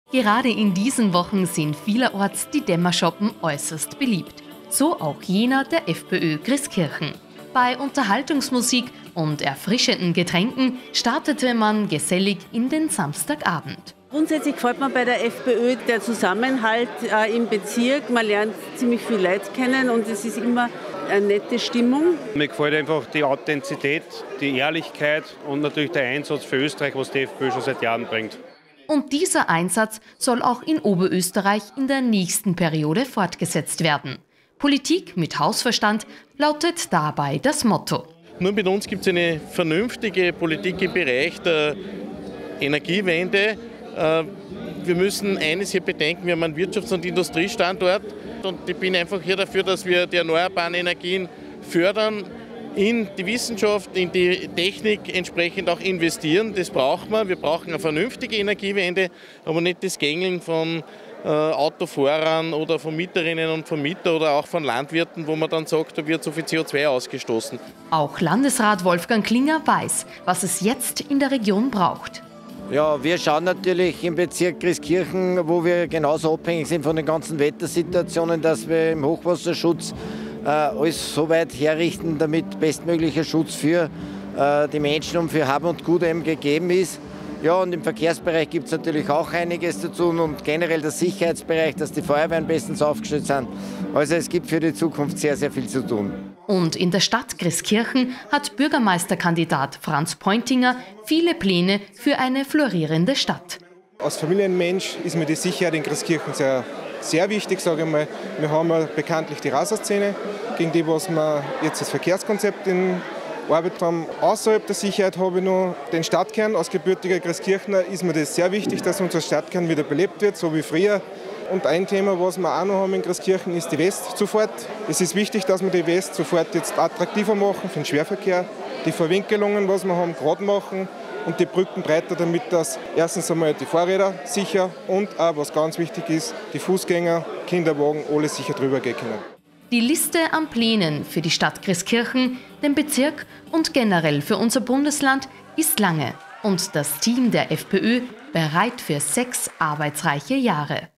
Grieskirchen: Geselliger Dämmerschoppen der FPÖ Grieskirchen
LIVE-Musik, erfrischende Getränke und beste Stimmung. All' das bot der FPÖ Dämmerschoppen in Grieskirchen. Darüber hinaus wurden den Gästen die Wahlziele auf kommunaler und Landesebene näher gebracht.